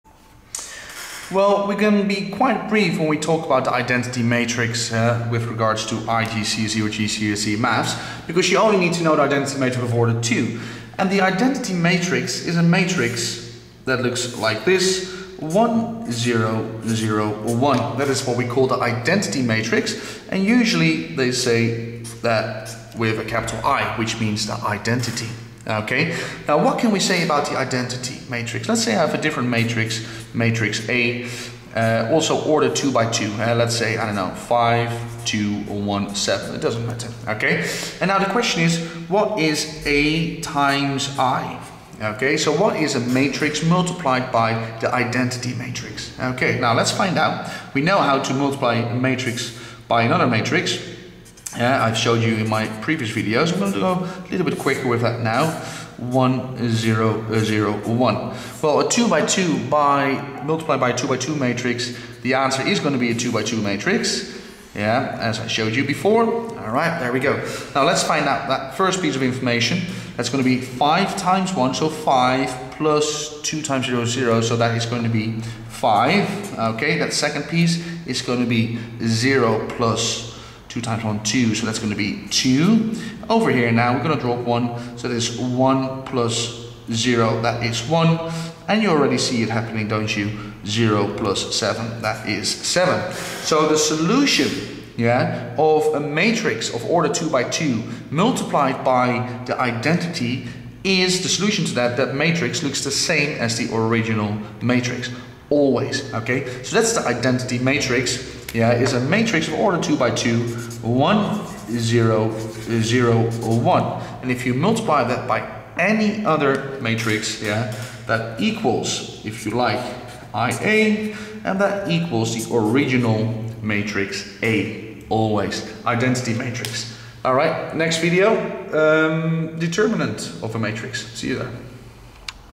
What is the identity matrix - Maths Help - ExplainingMaths.com IGCSE GCSE Maths
Understand what the identity matrix is by looking at this example question.  I will explain to you what the identity matrix is so you can pass your maths exams too.  Check for more maths help and FREE resource, activities and worksheets. This maths video will explain this IGCSE and GCSE mathematical topic to you by providing example questions.  You can use these maths activities for your math revision to do better on your IGCSE GCSE exam paper.